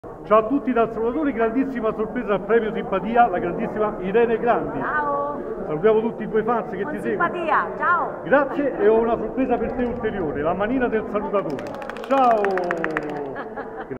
Premio Simpatia 2017 a Irene Grandi che saluta i fans col Salutatore
Premio Simpatia 2017 a Irene Grandi che saluta i fans col Salutatore che gli dona la famosa Manina del Salutatore.
➜ FACEBOOK IL SALUTATORE:
➜ PAGINA